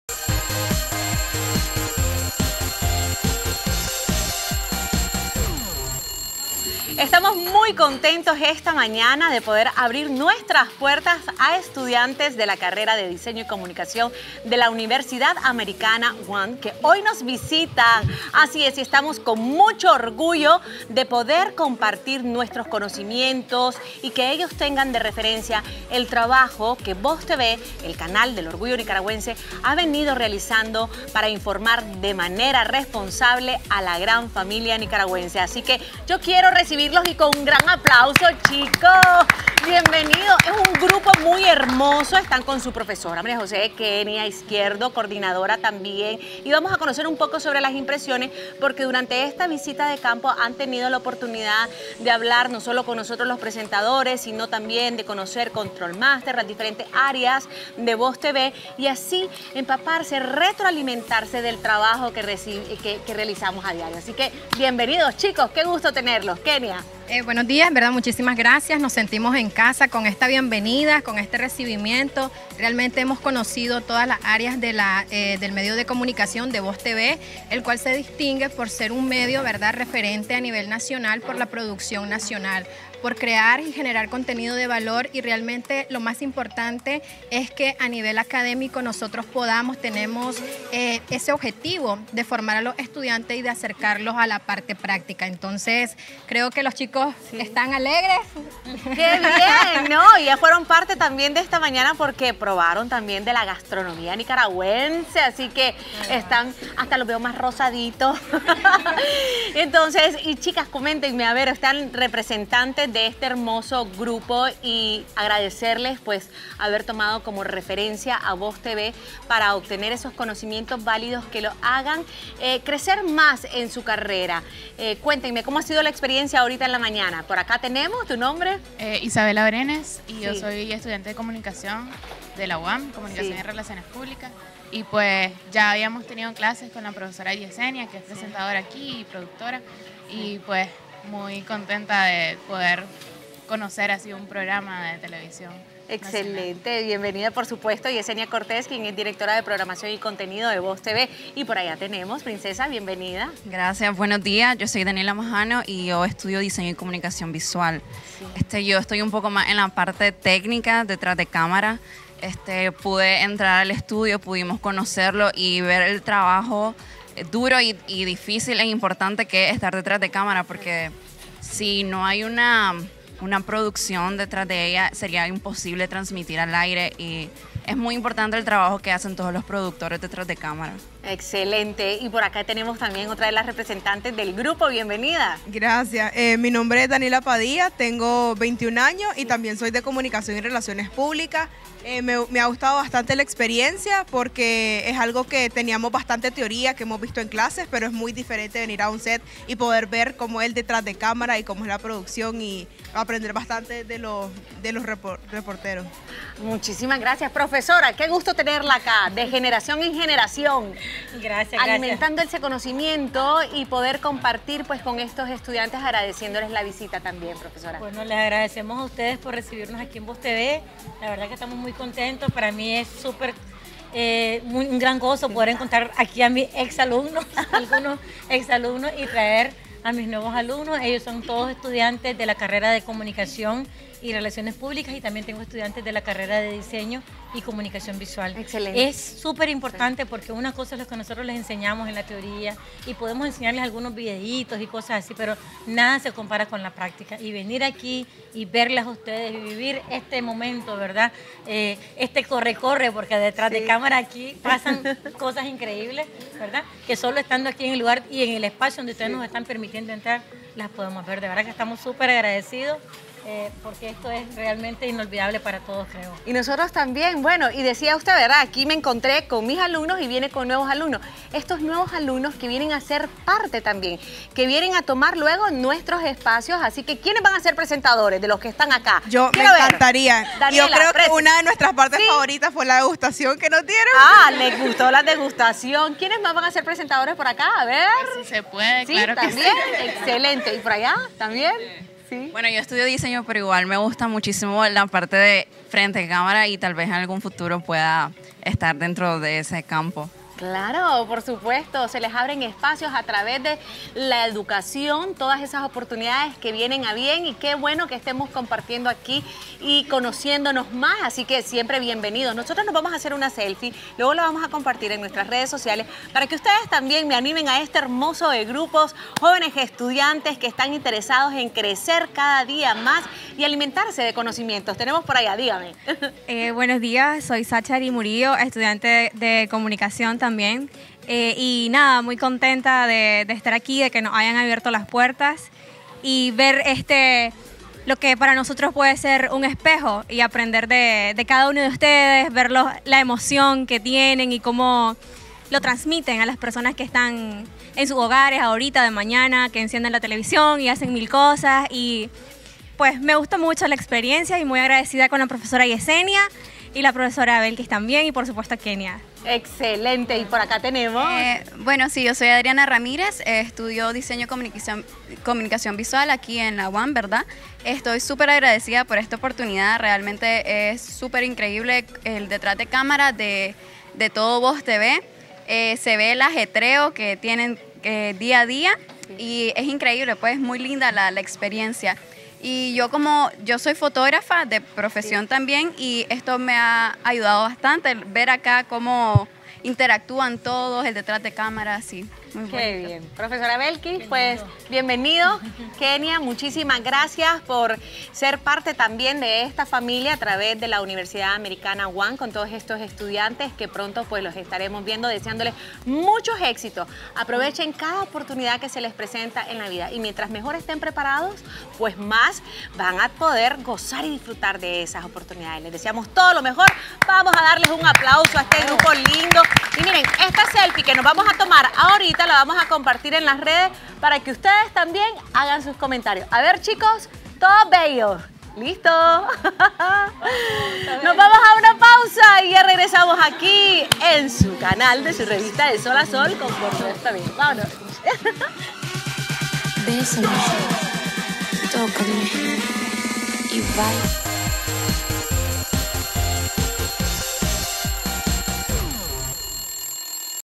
Estudiantes de Diseño y Comunicación de la UAM visitan instalaciones de VOSTV
👉🏻 Hoy visitaron las instalaciones de VOSTV estudiantes de la Universidad Americana de las carreras de
✅Comunicación y Relaciones Públicas.
✅Diseño y Comunicación Visual.
Los universitarios conocieron de cerca la forma de hacer televisión en dicho medio 📺